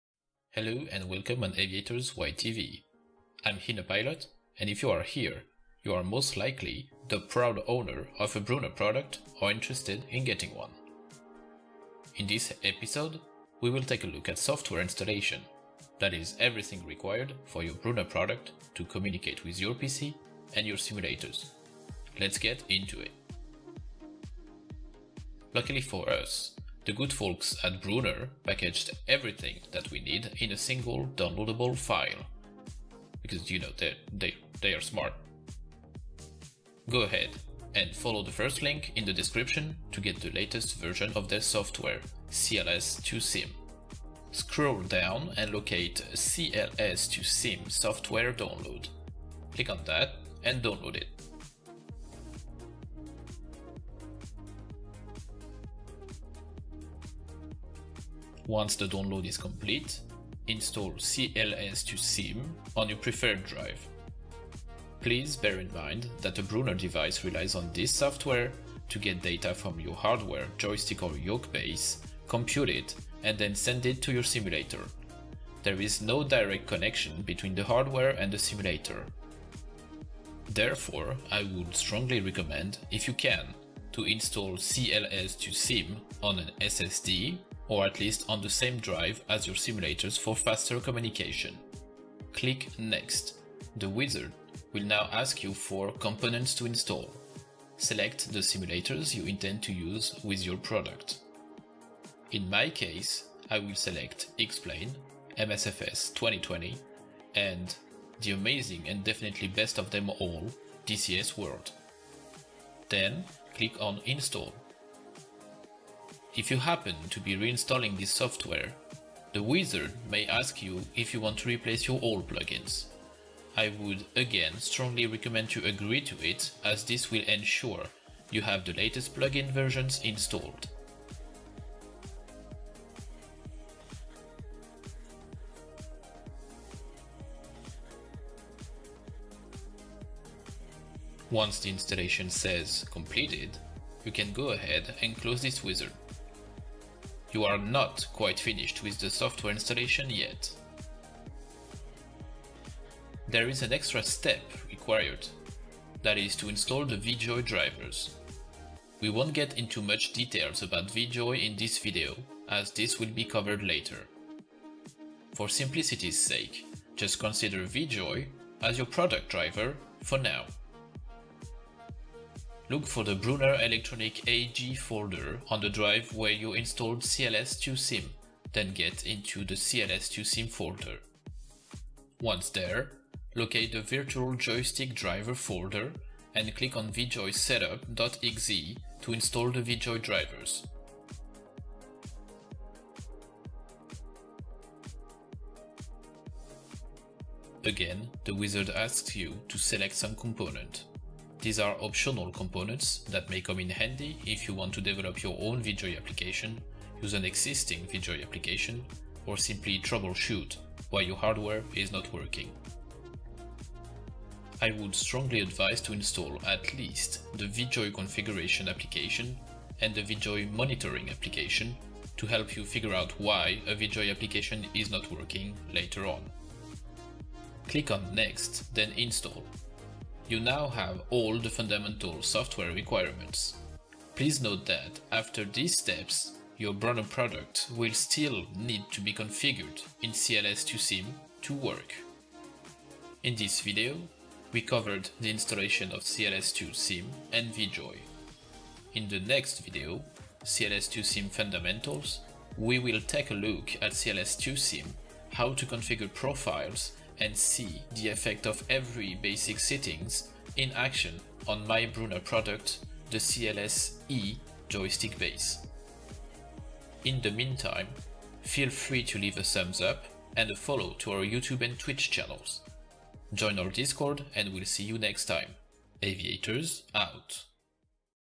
[AviatorsYTV] Brunner Products Guides - Software Installation
Introduction video to the Brunner Products Guides series. In this video we will take care of the software installation part.